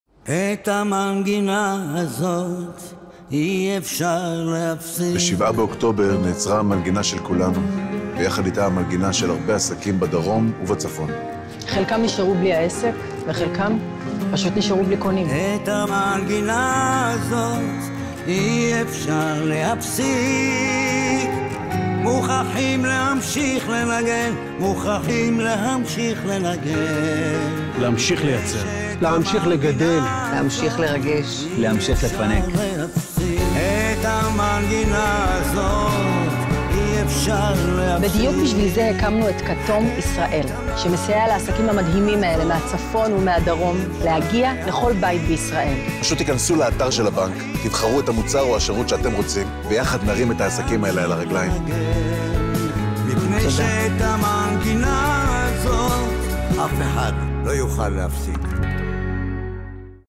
Noifeld handmade music | Mizrahi bank 2023
מילים : עודד פלדמן
לחן: יאיר רוזנבלום
עיבוד והפקה מוזיקלית: שמוליק נויפלד ורועי נויפלד
טיפול בזכויות יוצרים Noifeld handmade music
משרד פרסום: ראובני פרידן
במאי: רועי רז
שירה: בעז שרעבי